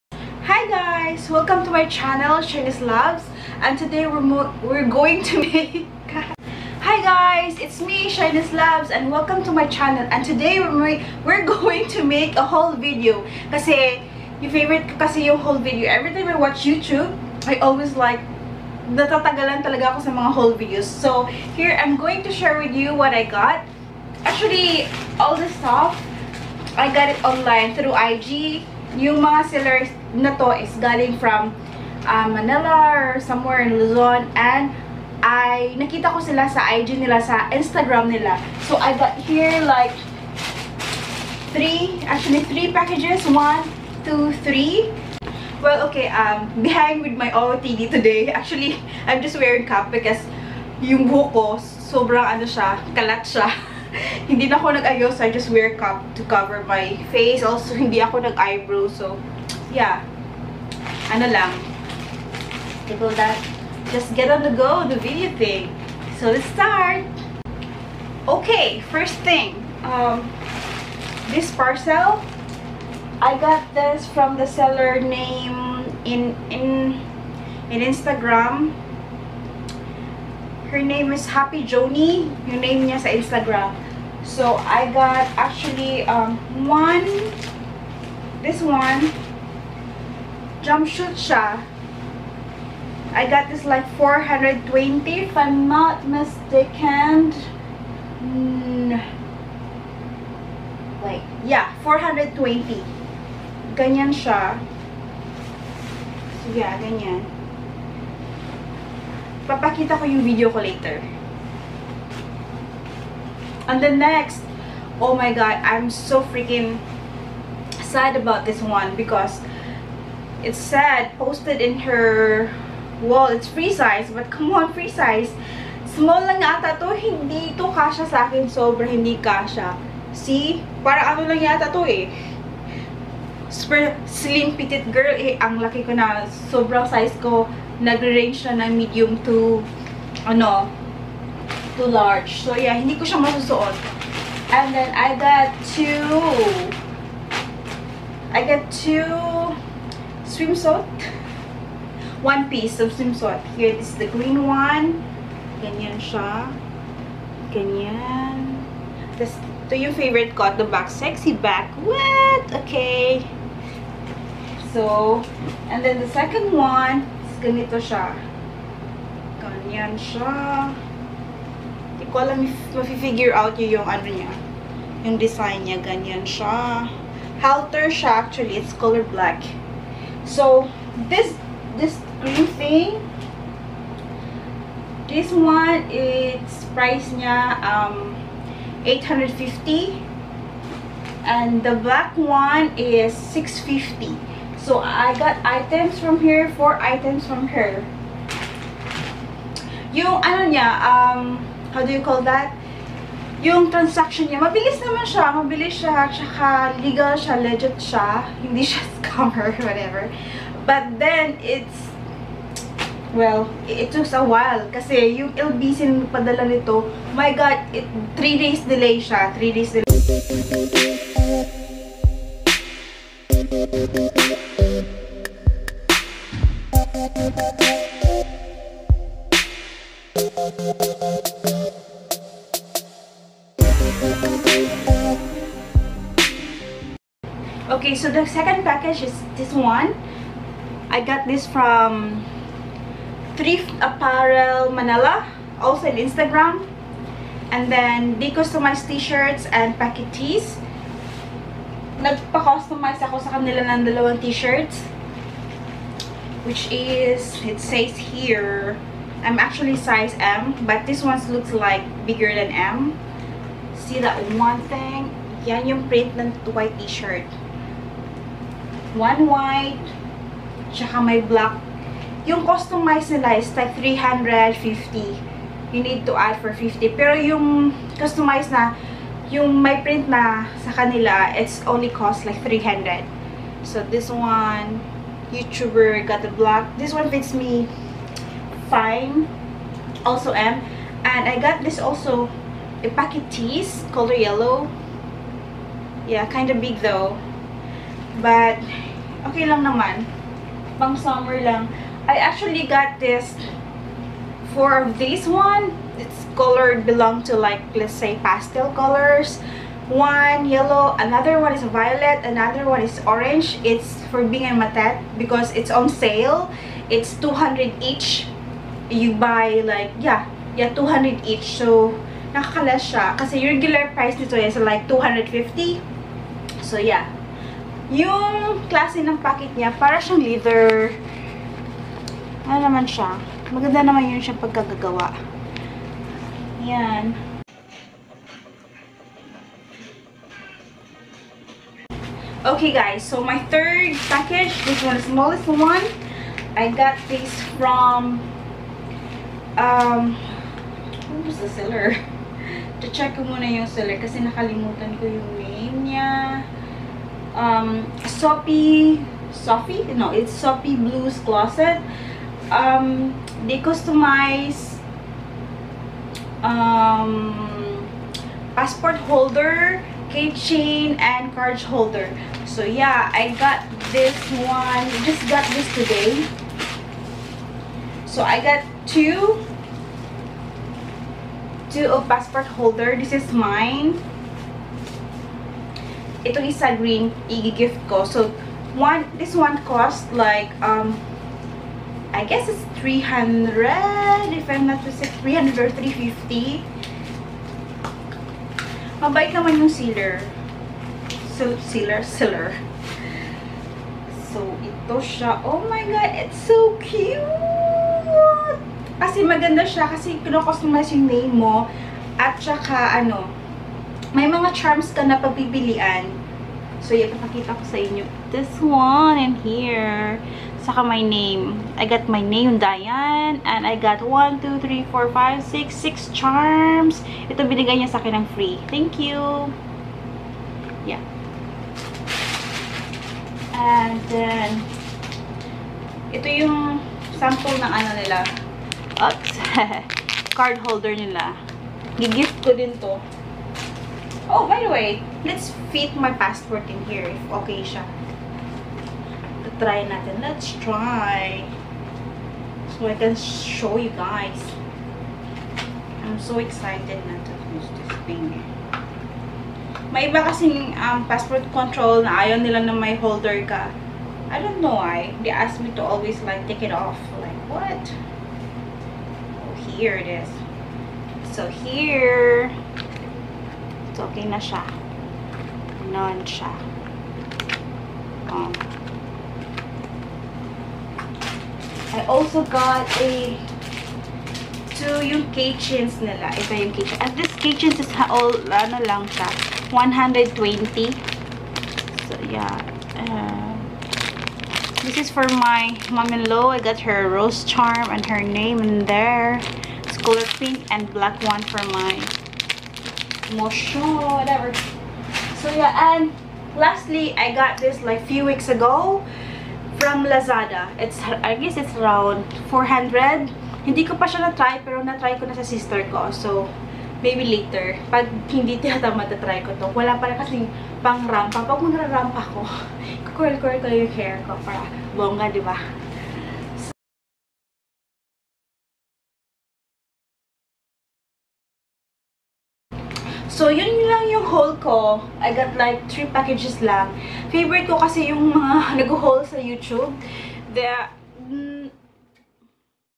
Instagram Online HAUL | First Ever HAUL! | SHYNESSLAVZ 💚
Heyya! LOVIES! This is my first ever Haul Video for ya! Purchased all stuff through online "Instagram Sellers". When buying items online please please make a background check / prof of any transactions and review from past buyers to be sure it's legit. And I promise to improve my talkings, back draft decor and lighting nextime! 😄

Beware of Online Scammers merchant!

Camera I use on my Vlogs:
Canon N2 | GoPro Hero4 Silver

I am Always grateful and appreciate that you stopby to my Channel!

Love & Hugs,
Lavz ❤